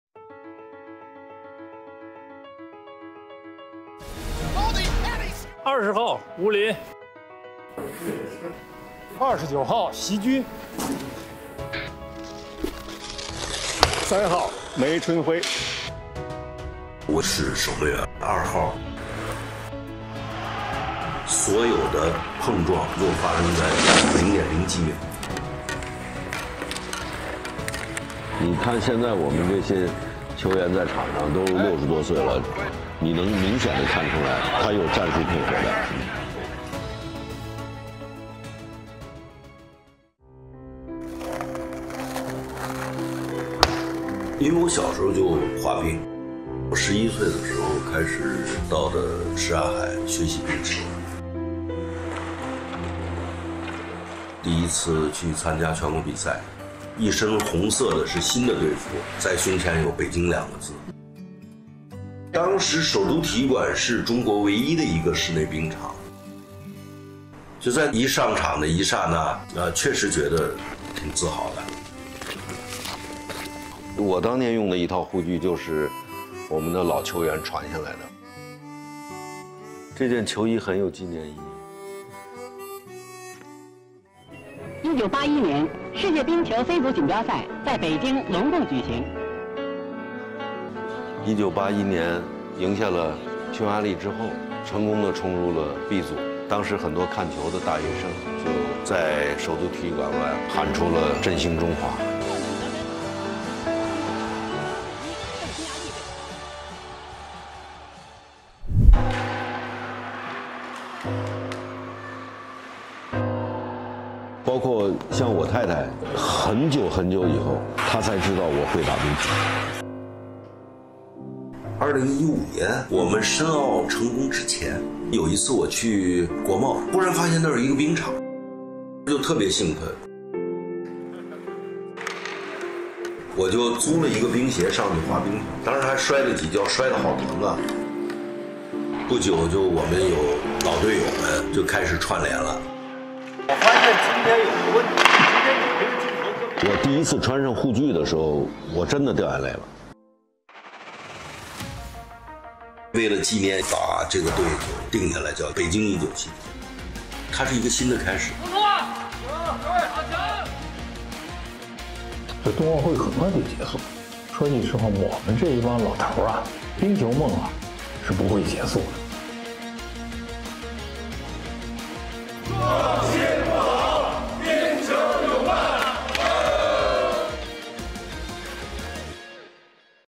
The Ice Hockey Boys are still "Fast and Furious" after 40 years!
The Beijing 1979 Ice Hockey team is made up of players aged over 60, many of which were in an amateur team in the 1970s. Disbanded in 1979, the team saw its members leave and start pursuing different careers.

In 2015, Beijing won the bid to host the 2022 Winter Olympics. In the same year, the Beijing 1979 team restarted training after over 40 years. It’s never too late to chase your ice hockey dreams!